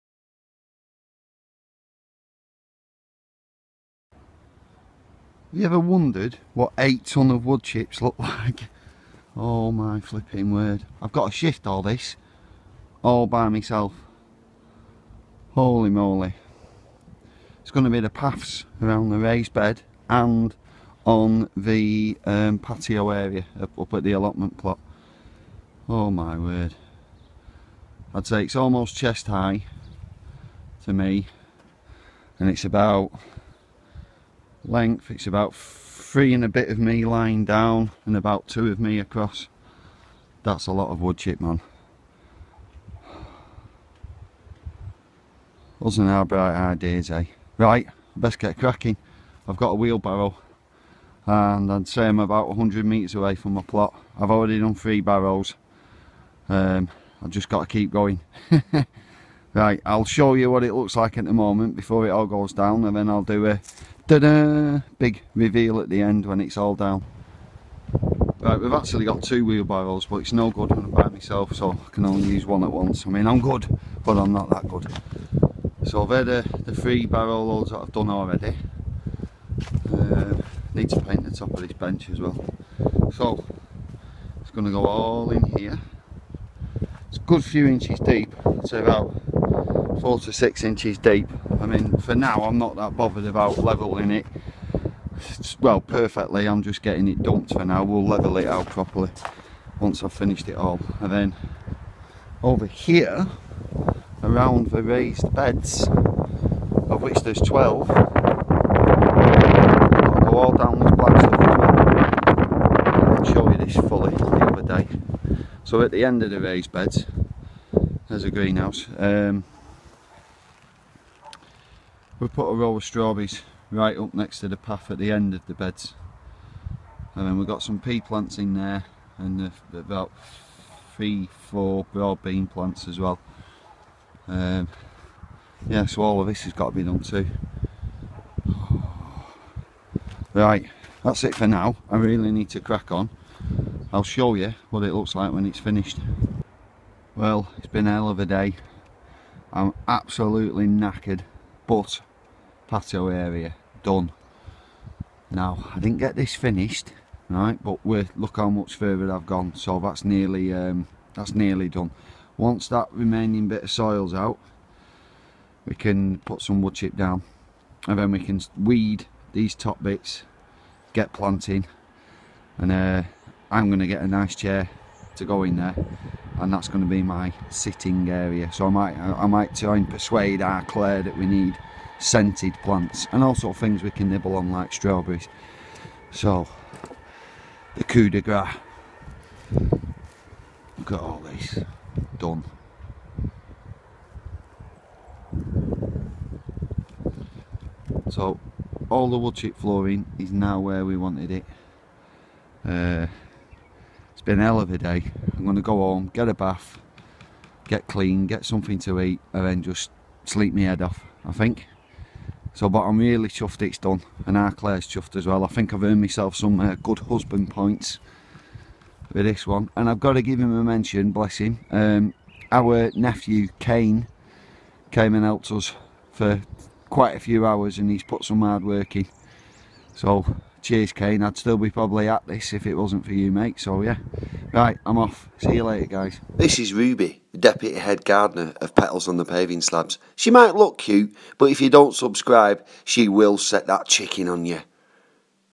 The path to happiness on our allotment plot
8 tonnes of wood chip, done.